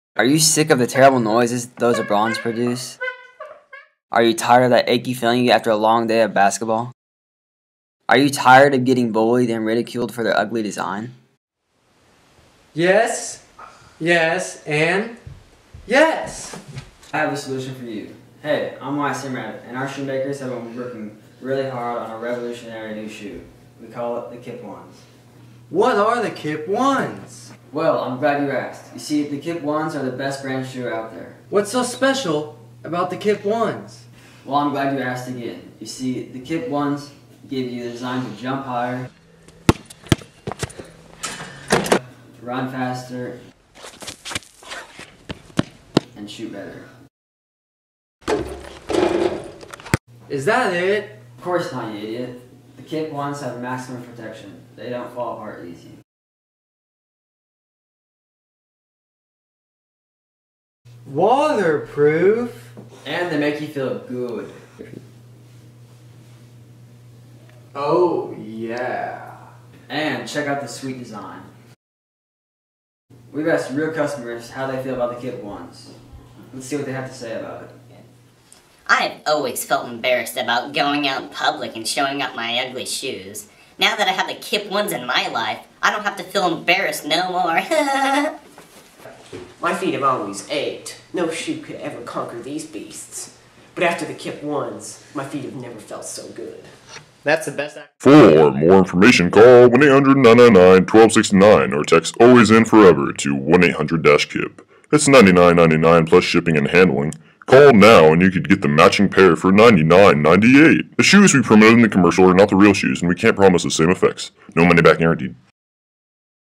Kip 1's Commercial
2 years ago we filmed and poorly edited a crappy commercial parody. This is it.